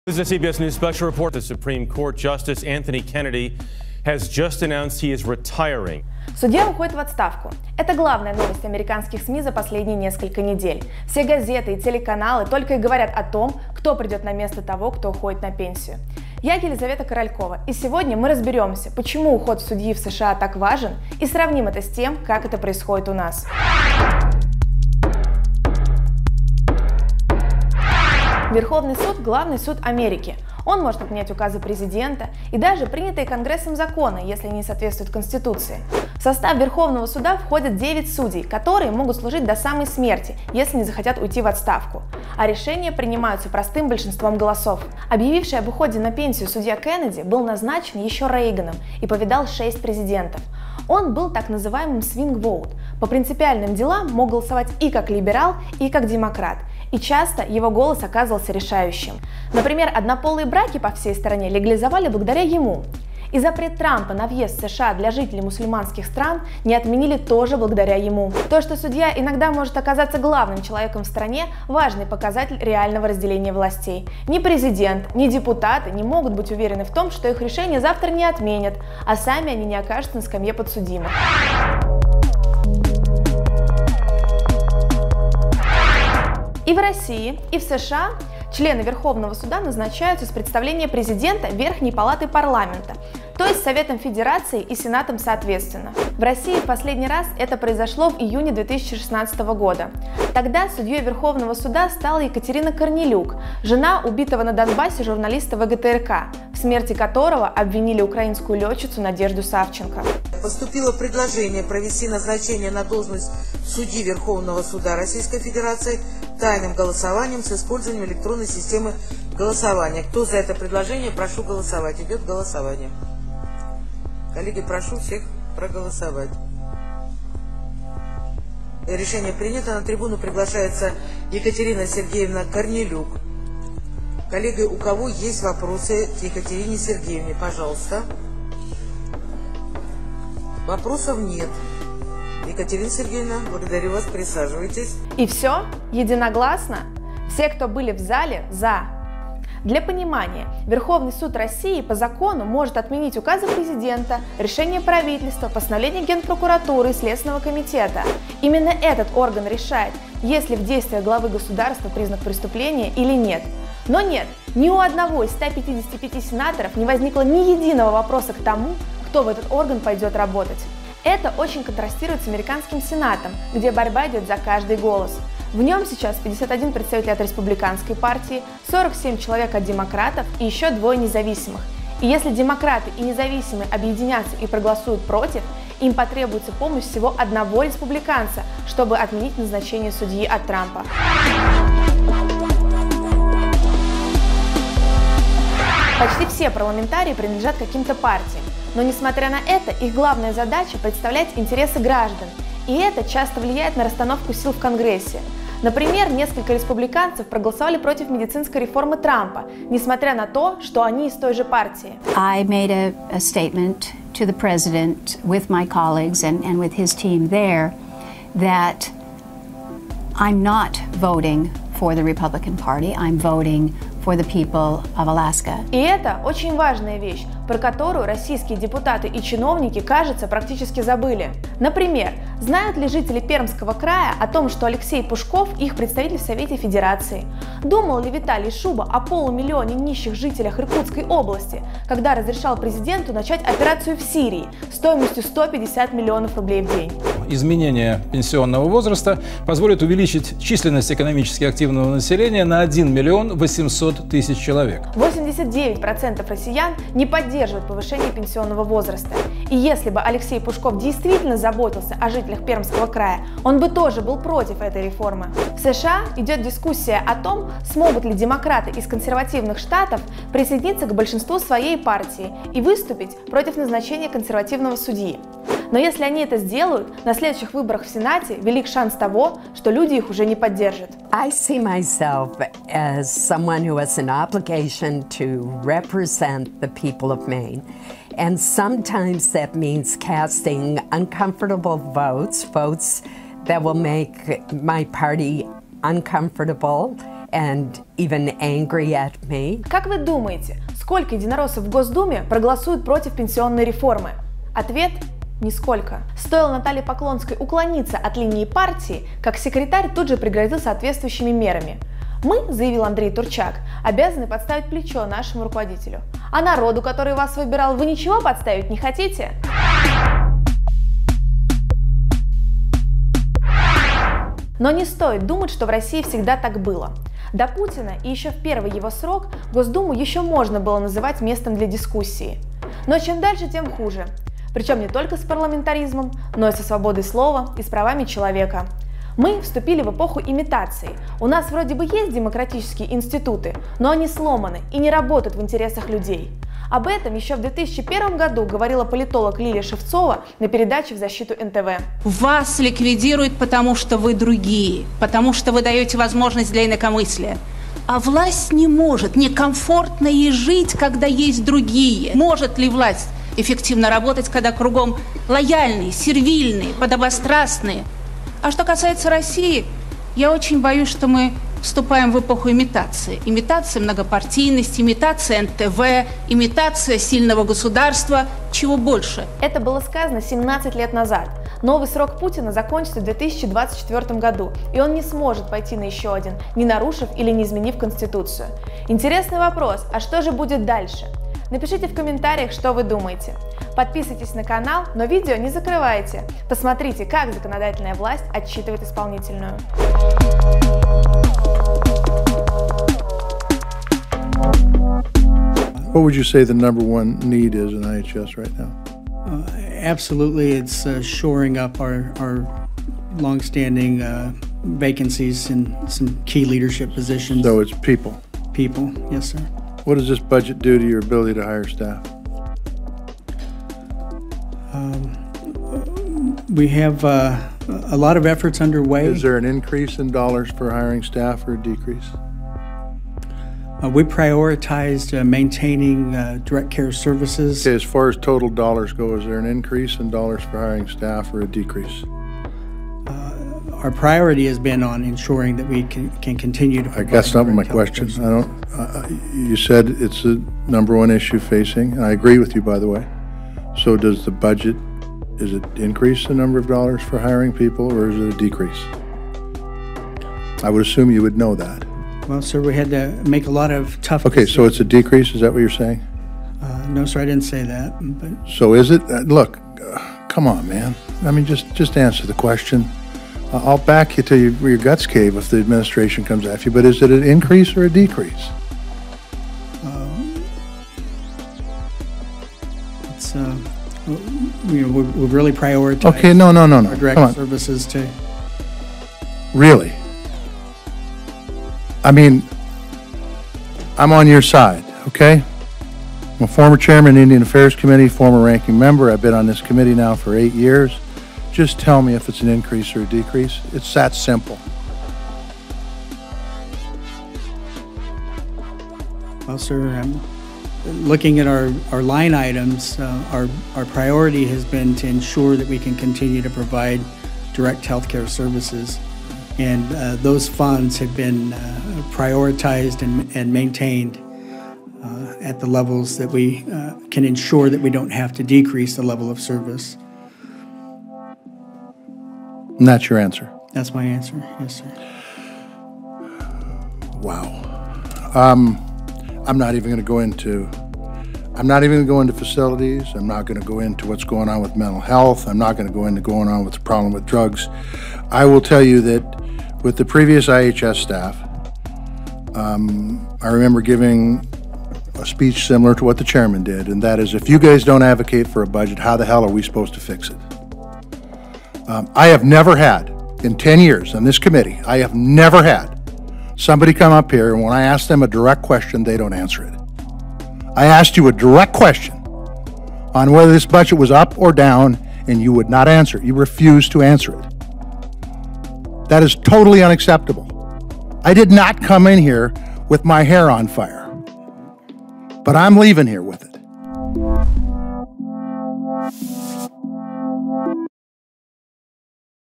ЧТО ПУТИН СДЕЛАЛ С РОССИЕЙ
Путин и Трамп встретились в Хельсинки, а Елизавета Королькова решила сравнить, как работают демократии в России и США. Противостояние двух стран — в сути политических систем.